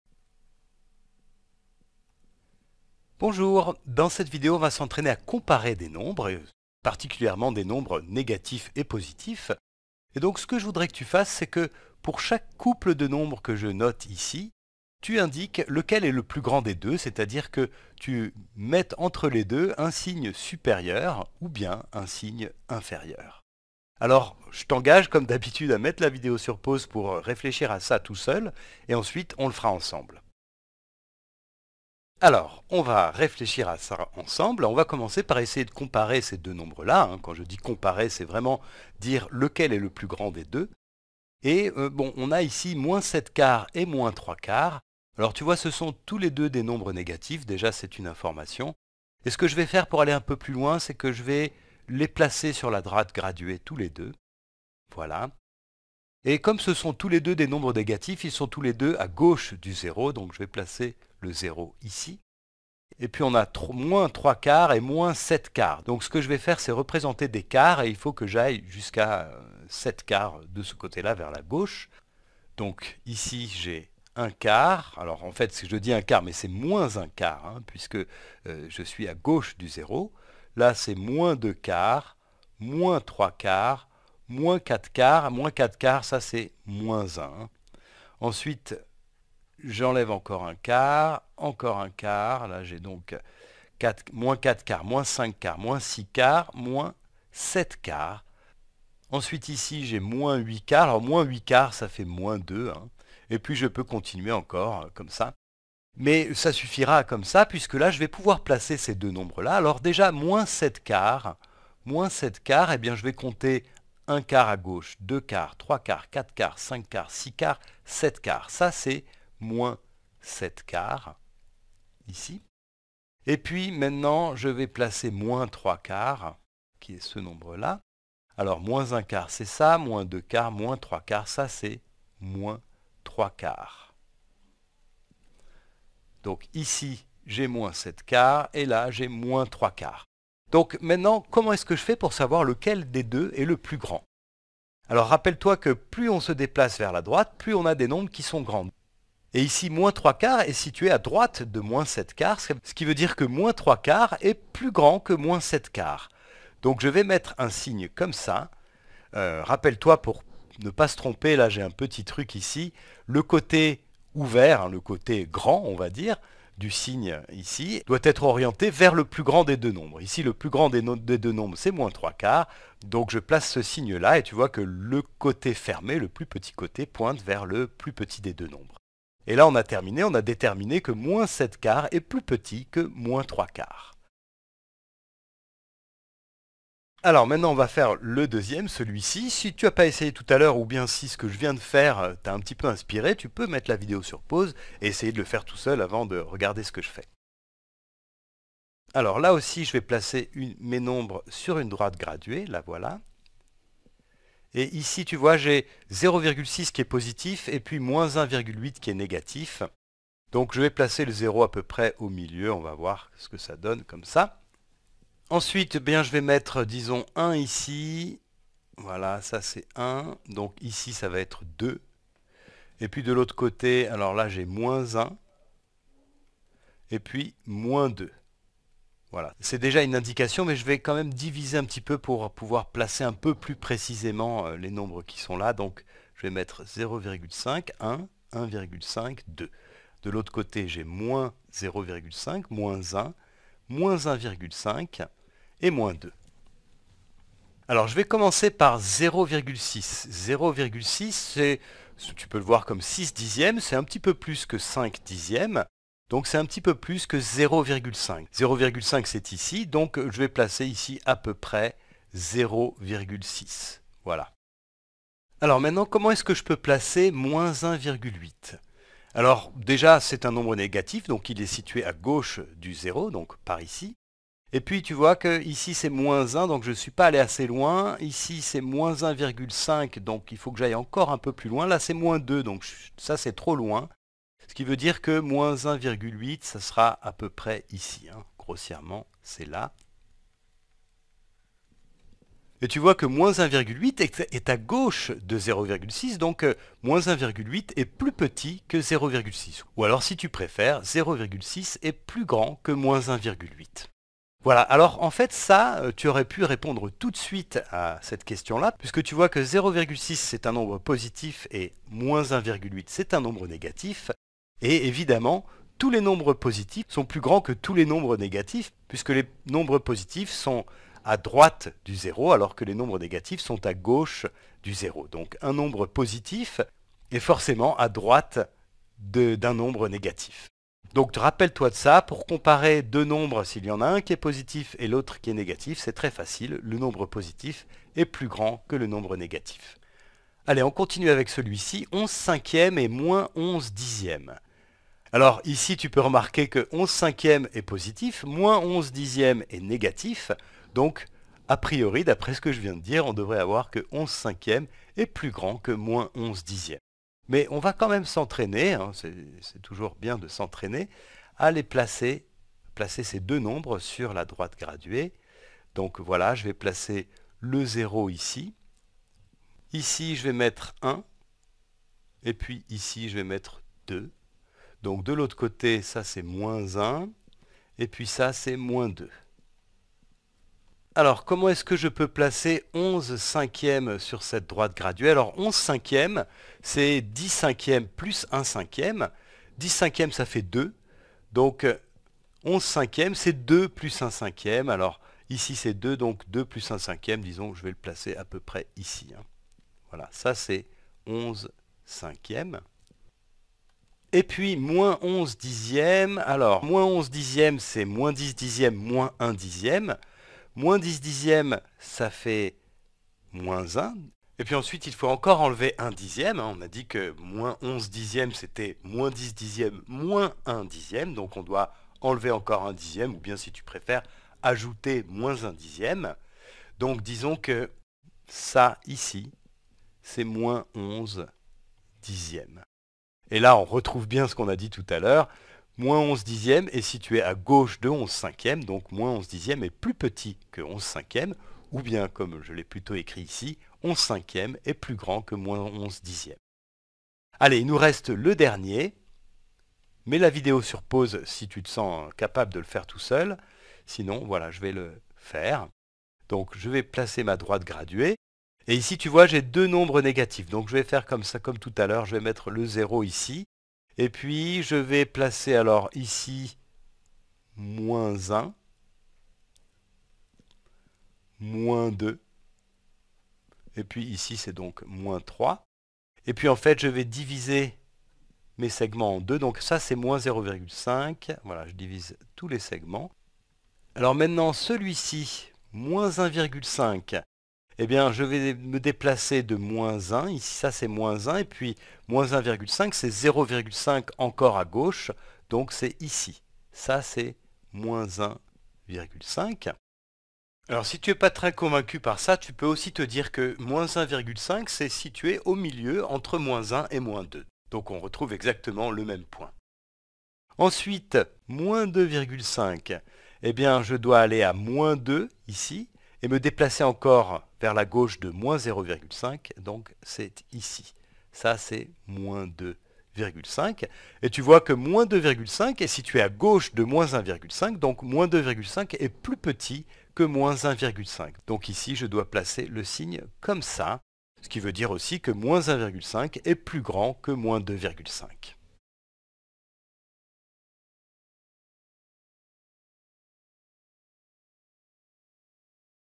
Comparer des nombres rationnels à l'aide de la droite numérique
On compare des paires de nombre rationnels en les plaçant sur la droite numérique.